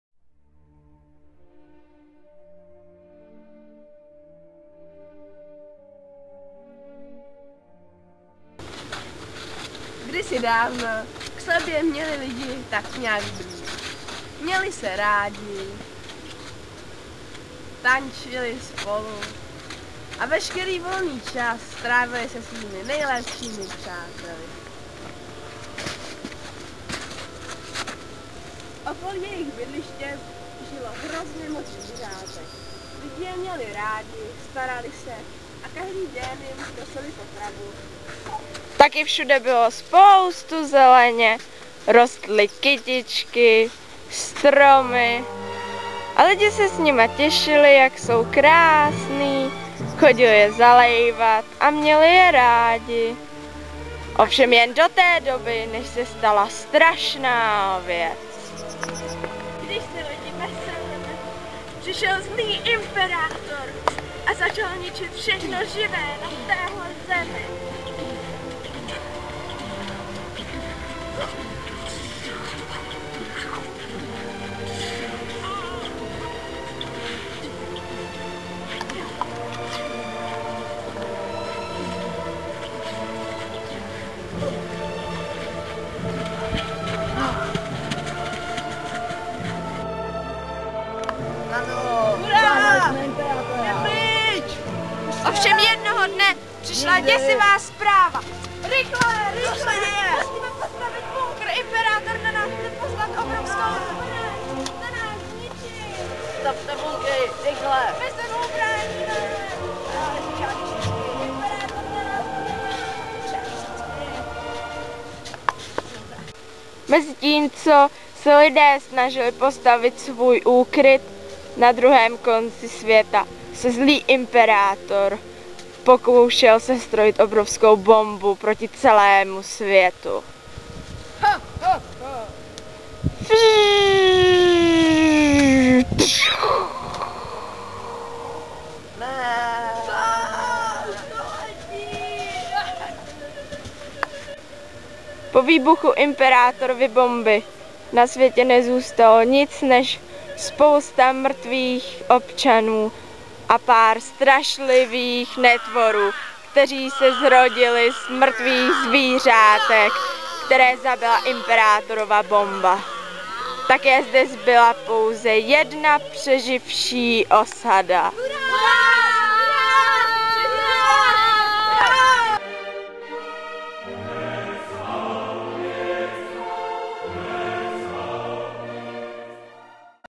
Kryt 129
Intro k celoroční hře s postapokalyptickou tématikou vytvořené samotnými dětmi.